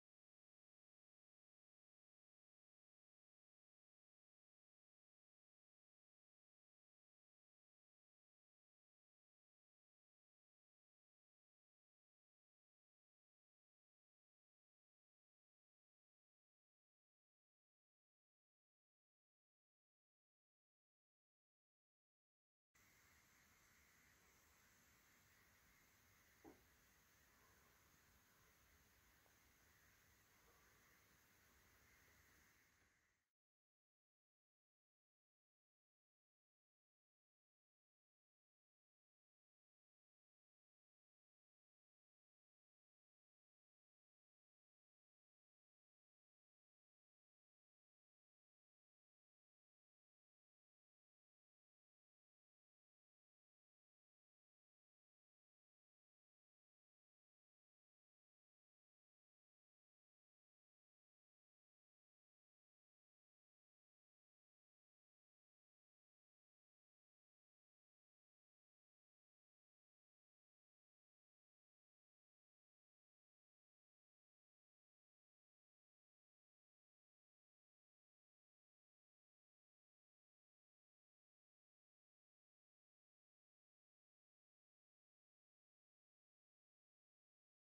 3d printed Hydrofoil for Windsurf Foiling
Windfoil with 3d printed core and Carbon Lamination.  A Easy Process to become hight strength Carbon Parts.